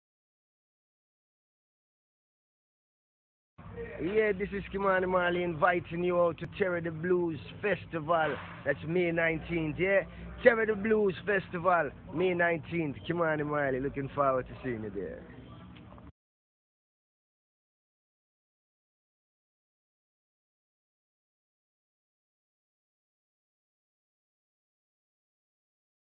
KyMani Marley message video to You - TDB2018
Ky-Mani Marley en concert : samedi 19 mai au Festival Terre de Blues de Marie-Galante.
Points de vente : boutiques du réseau Allmol (Microforce et Sébastiano) ET Office du Tourisme de Marie-Galante (ouverture programmée)

Tous à Marie-Galante du 18 au 21 mai 2018 pour 4 jours de festivités, concerts, animations et soirée clubbing !

Une 19ème édition "Fanm Doubout'" sous le marrainage de Marie-Hélèna Laumuno.